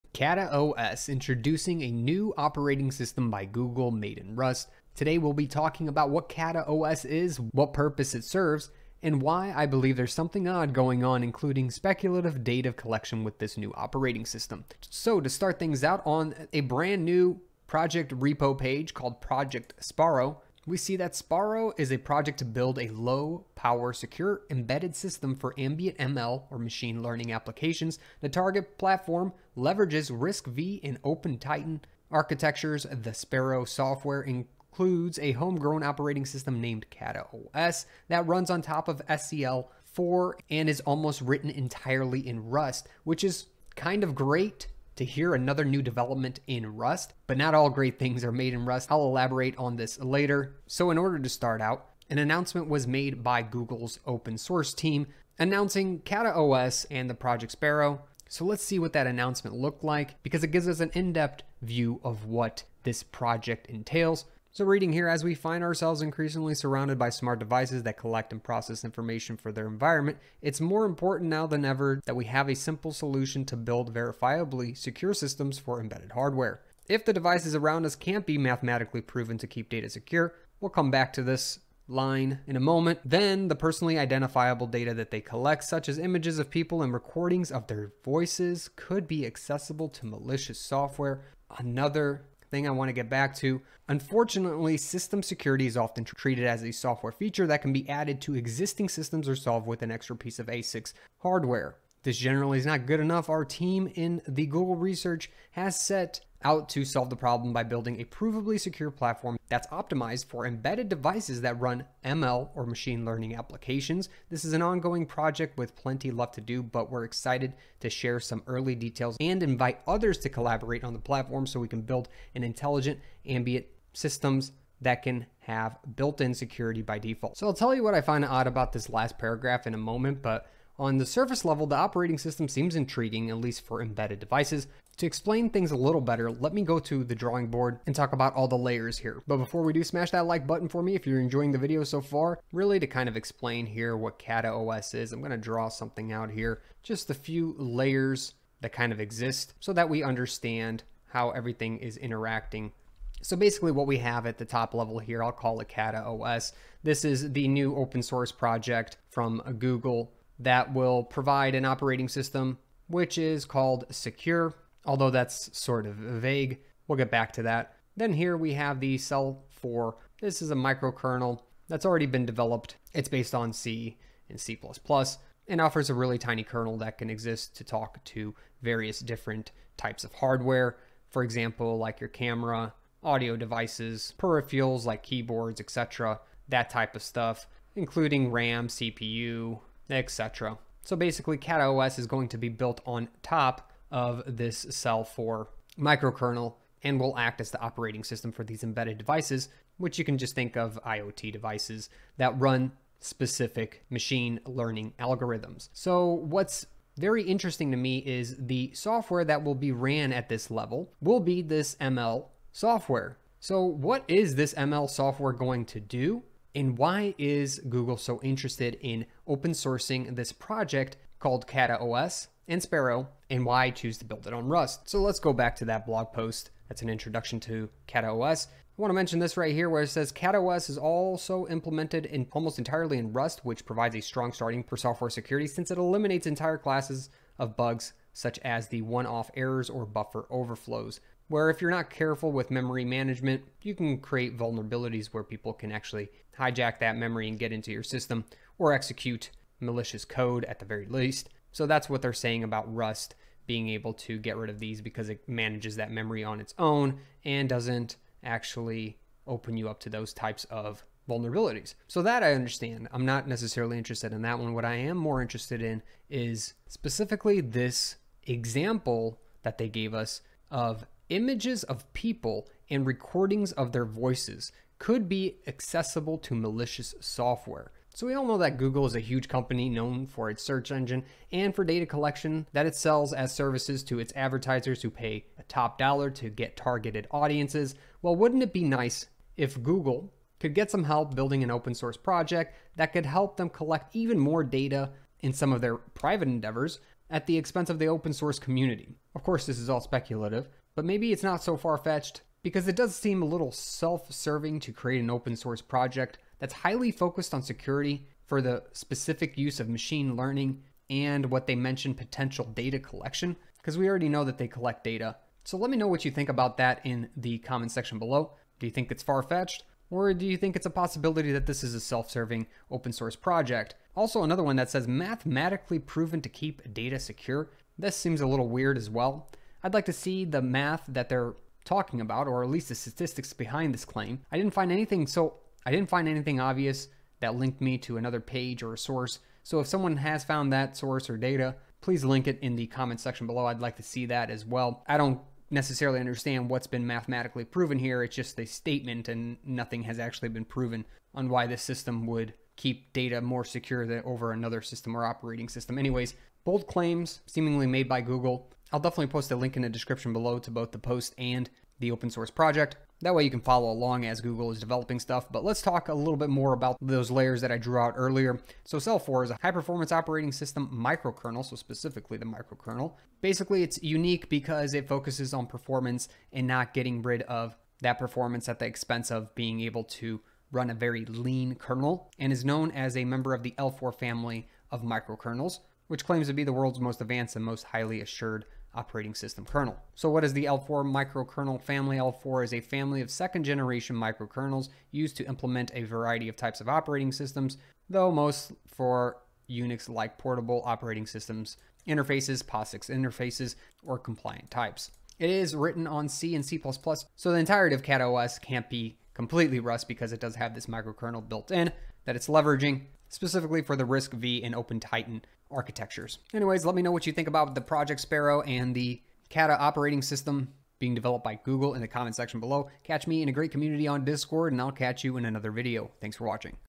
Will this RUST based OS collect more of our data? (KataOS by Google)
KataOS is an Operating System built on RUST meant for Machine Learning (ML) applications that's been open sourced by Google. Let's check out what KataOS is and what the Machine Learning maybe used to do. Will this Rust based Machine Learning OS collect more of our data?